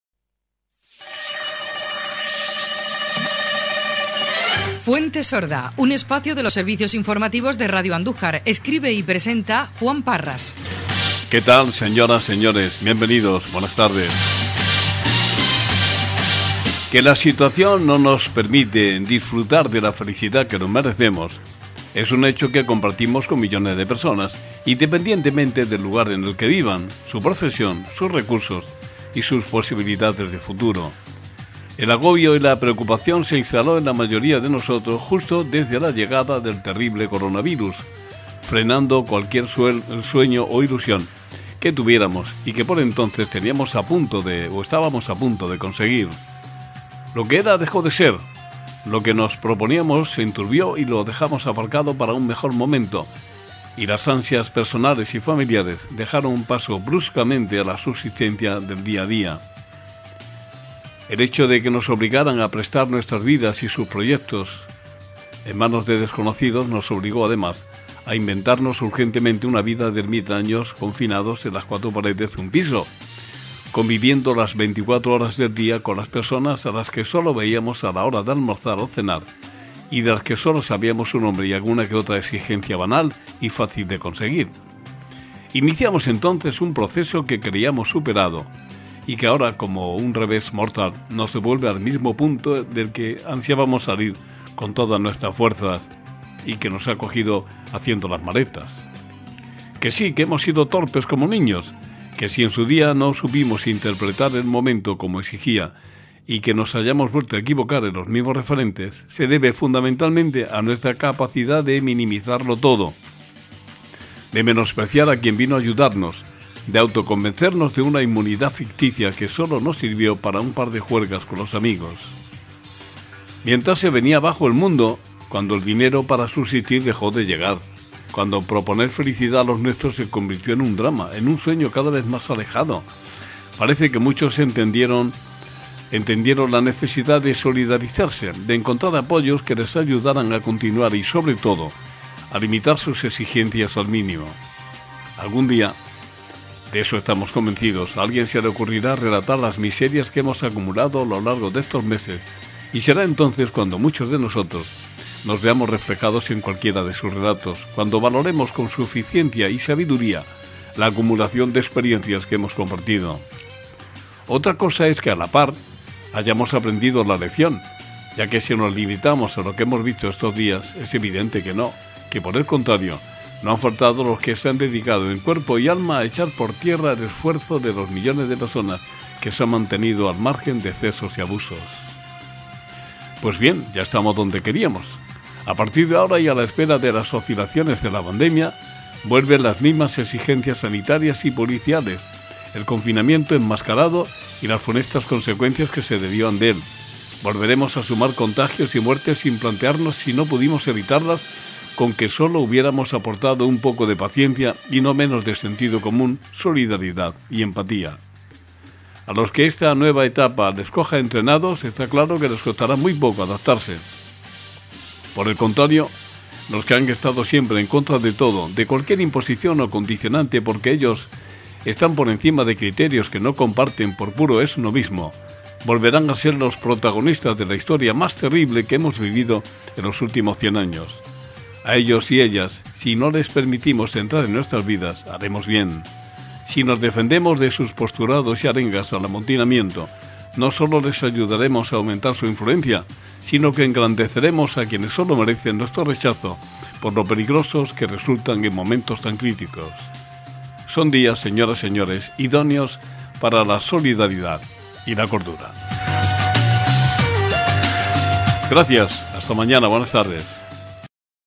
VUELVEN LOS FANTASMAS
FUENTE SORDA
Editorial de RADIO ANDÚJAR
Escribe Juan Parras García

Edición 28/10/20

Que la situación no nos permite disfrutar de la felicidad que nos merecemos es un hecho que compartimos con millones de personas, independientemente del lugar en el que vivan, su profesión, sus recursos y sus posibilidades de futuro. El agobio y la preocupación se instaló en la mayoría de nosotros justo desde la llegada del terrible coronavirus, frenando cualquier sueño o ilusión que tuviéramos y que por entonces estábamos a punto de conseguir. Lo que era dejó de ser; lo que nos proponíamos se enturbió y lo dejamos aparcado para un mejor momento, y las ansias personales y familiares dejaron paso bruscamente a la subsistencia del día a día. El hecho de que nos obligaran a entrregar nuestras vidas y sus proyectos en manos de desconocidos nos obligó, además, a inventarnos urgentemente una vida de ermitaños confinados en las cuatro paredes de un piso, conviviendo las veinticuatro horas del día con personas a las que solo veíamos a la hora de almorzar o cenar y de las que solo sabíamos su nombre y alguna que otra exigencia banal y fácil de conseguir. Iniciamos entonces un proceso que creíamos superado y que ahora, como un revés mortal, nos devuelve al mismo punto del que ansiábamos salir con todas nuestras fuerzas y que nos ha cogido haciendo las maletas.

Que sí, que hemos sido torpes como niños, que si en su día no supimos interpretar el momento como exigían las circunstancias y que nos hayamos vuelto a equivocar en los mismos referentes, se debe fundamentalmente a nuestra capacidad de minimizarlo todo, de menospreciar a quien vino a ayudarnos, de auto convencernos de una inmunidad ficticia que solo nos sirvió para un par de juergas con los amigos… Mientras se venía abajo el mundo, cuando el dinero para subsistir dejó de llegar, cuando proponer felicidad a los nuestros se convirtió en una utopía, en un sueño cada vez alejado, parece que muchos entendieron la necesidad de solidarizarse, de encontrar apoyos que les ayudaran a continuar y, sobre todo, a limitar sus exigencias. Algún día, de eso estamos convencidos, a alguien se le ocurrirá relatar las miserias que hemos acumulado a lo largo de estos meses y será entonces cuando muchos de nosotros nos veamos reflejados en cualquiera de sus relatos y cuando valoremos con suficiencia y sabiduría la acumulación de experiencias que hemos compartido. Otra cosa es que, a la par, hayamos aprendido la lección, ya que, si nos limitamos a lo que hemos visto estos días, es evidente que no, que, por el contrario, no han faltado lo que se han dedicado en cuerpo y alma en echar por tierra es esfuerzo de los millones de personas que se han mantenido al margen de excesos y abusos.

Pues bien, ya estamos donde queríamos. A partir de ahora, y a la espera de las oscilaciones de la pandemia, vuelven las mismas exigencias sanitarias y policiales, el confinamiento enmascarado y las funestas consecuencias que se derivan de él; volveremos a sumar contagios y muertes sin plantearnos si no pudimos evitarlas con que solo hubiéramos aportado un poco de paciencia y no menos de sentido común, solidaridad y empatía. A los que esta nueva etapa les coja entrenados, está claro que les costará muy poco adaptarse; por el contrario, los que han estado siempre en contra de todo, de cualquier imposición o condicionante porque ellos están por encima de criterios que no compartan por puro esnobismo, volverán a ser los protagonistas de la historia más terrible que hemos vivido en los últimos cien años. A ellos y ellas, si no les permitimos entrar en nuestras vidas, haremos bien. Si nos defendemos de sus postulados y arengas al amotinamiento, no solo evitaremos que aumente su influencia, sino que pondremos nombre y cara a quienes solo merecen nuestro rechazo por lo peligrosos que resultan en momentos tan críticos. Son días idóneos para la solidaridad y la cordura.